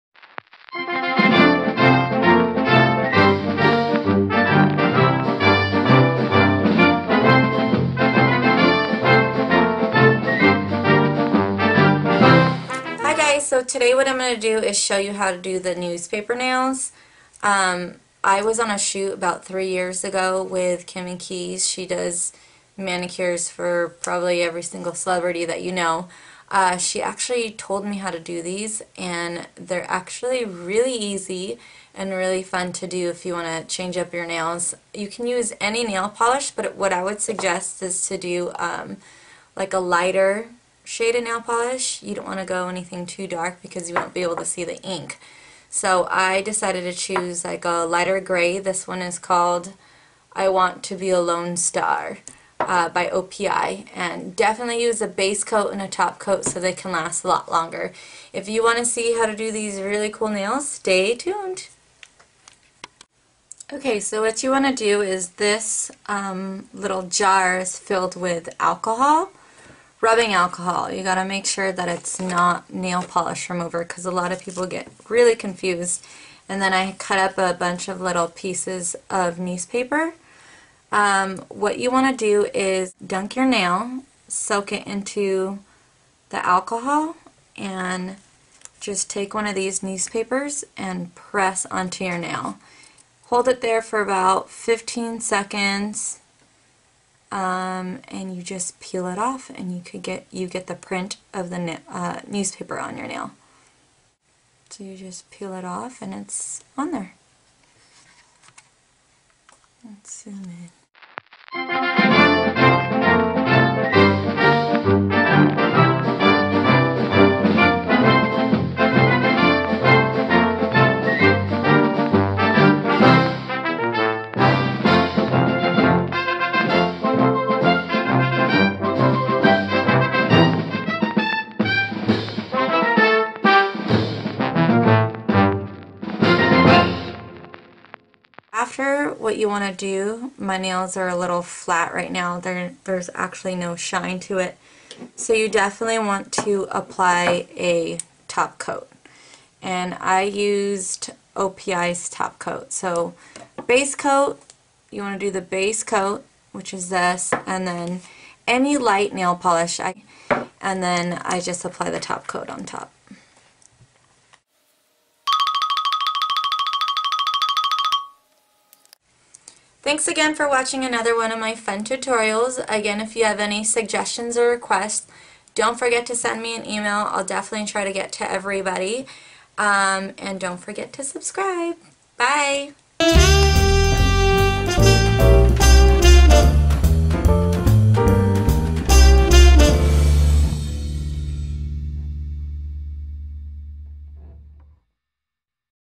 Newspaper Nail Art Tutorial; Alana Dawn
This is a simple tutorial on how to get newspaper print on your nails. A fun short video showing you some nail art. Have Fun!!!